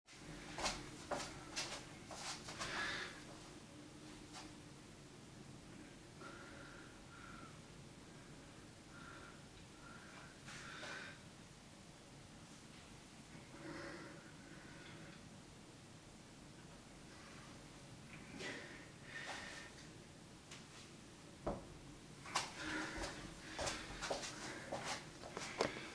12,5KG hex dumbbell pinch lift with the thumb, middle, ring and little finger at the weight marks
12,5KG hex dumbbell pinch lift with the thumb, middle, ring and little finger at the weight marks